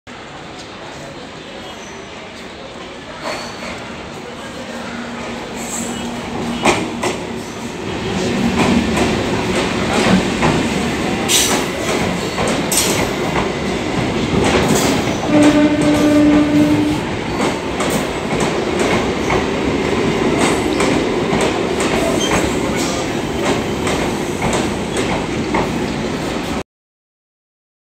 Bardhaman Rampurhat Memu passenger special train arriving Bolpur.
In this video, you will be watching the arrival of the train 03587 Bardhaman Rampurhat Memu passenger special at Bolpur station on a calm winter morning of January 2021.